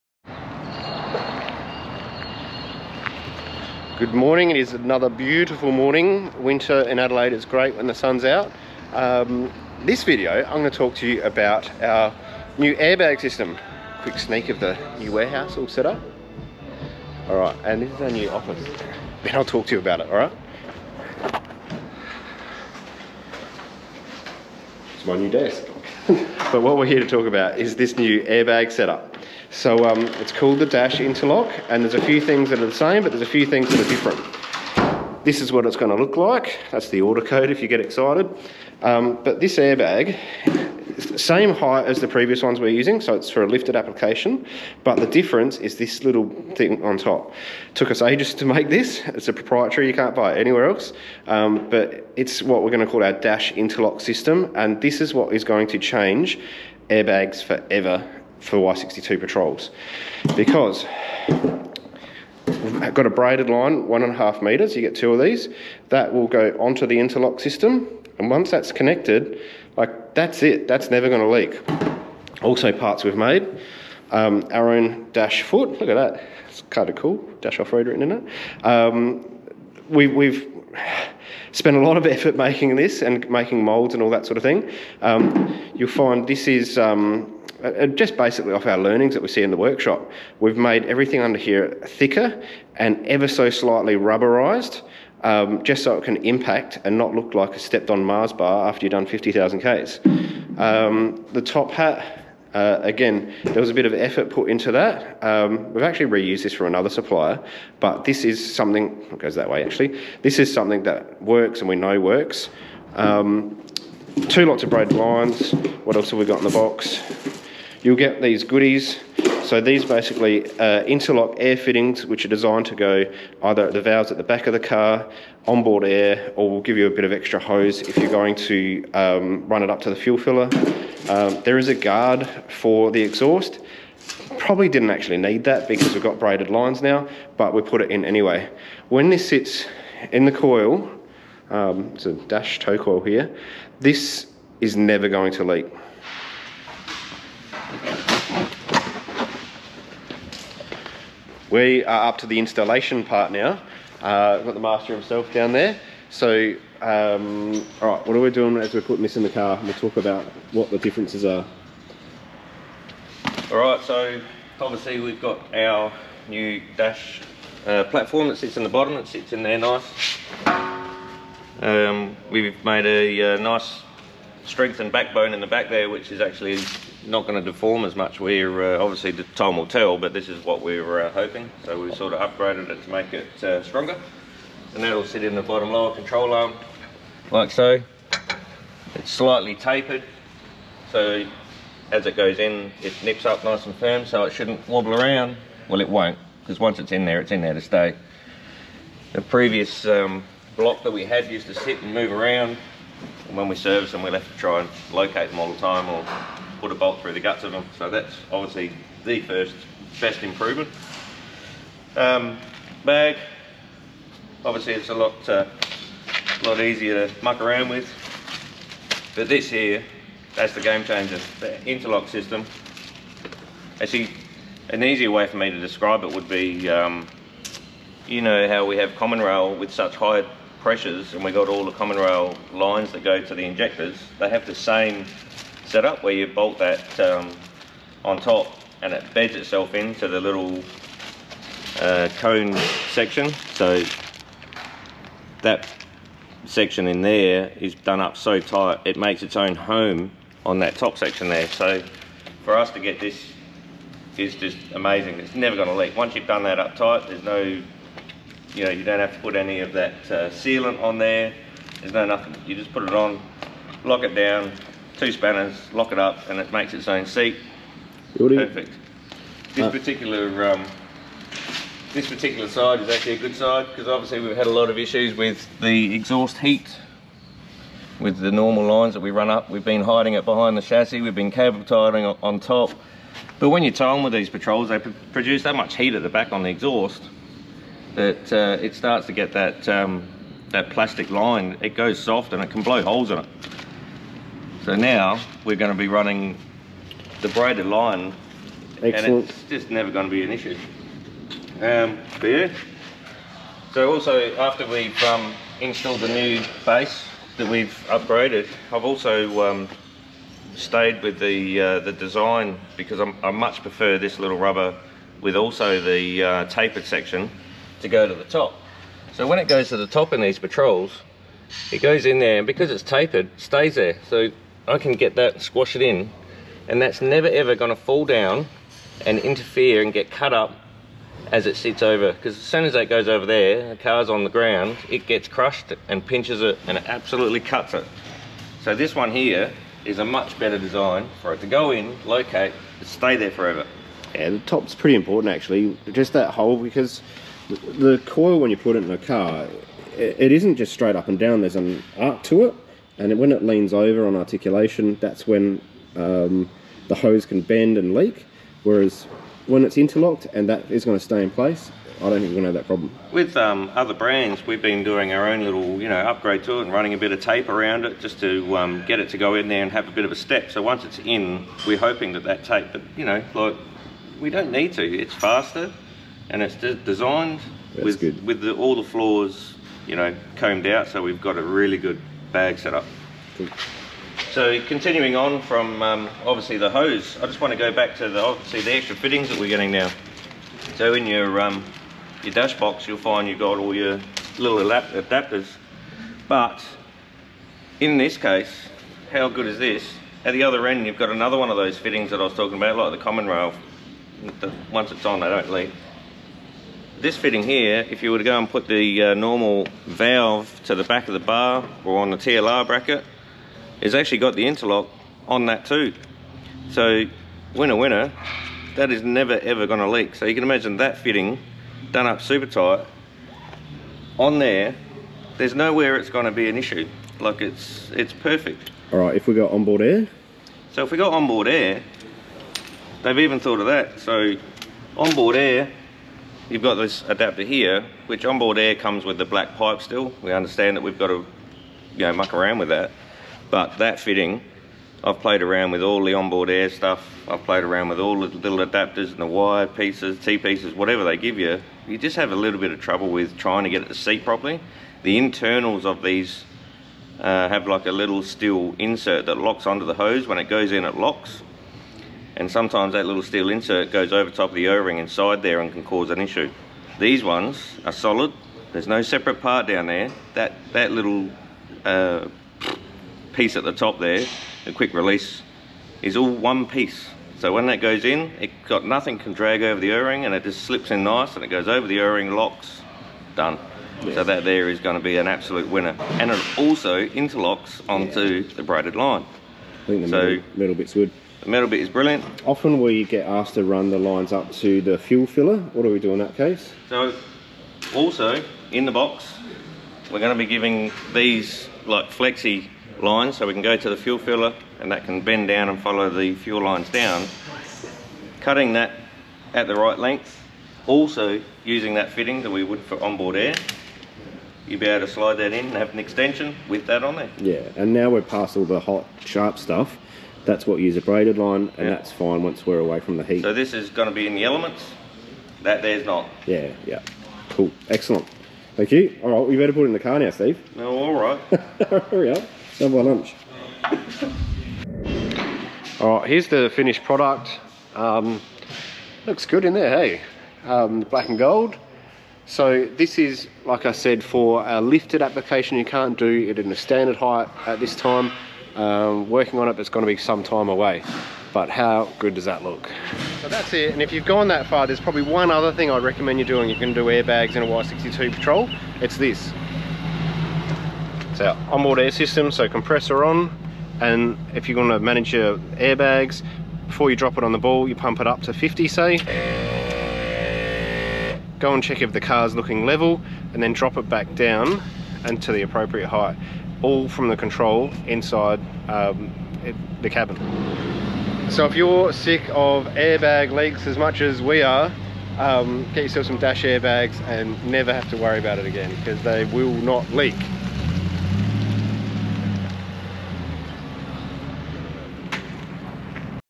Y62 Patrol Airbags That WON’T LEAK…. Ever!!!
This will change the way we think about airbags. Finally, an airbag that both increase spring rate, return height, AND WON’T LEAK!!!
Pretty much every Y62 Patrol needs airbags to assist the rear independent suspension in carrying unbolted mass and keeping the wheels aligned. This improves handling and tyre wear also. The problem has always been though, that airbags can leak. This can be very frustrating as a workshop owner so we have come up with our own airbag system using an “Interlock” system that positively locks the airbag to braided lines. This avoids heat issues or line and connection failure.
Have a watch and tell us what you think